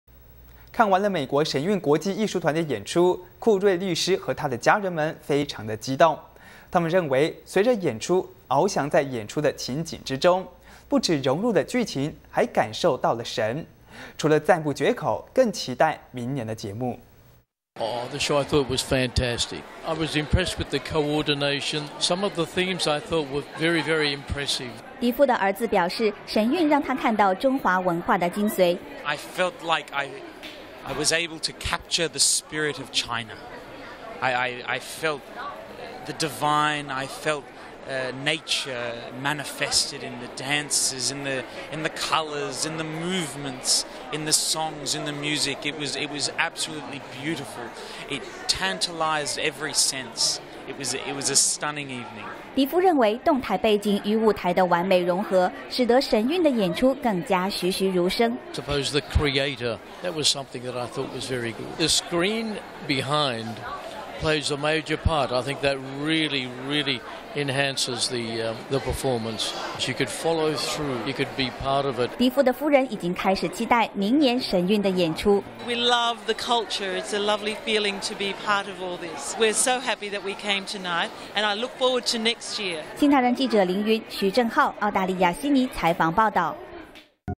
神韻帶領律師一家 漫遊其中
看完了美國神韻國際藝術團的演出，庫瑞律師和他的家人們，非常的激動，他們認為，隨著演出翱翔在演出的情景之中，不止融入了劇情還感受感受到了神。除了讚不絕口，更期待明年的節目。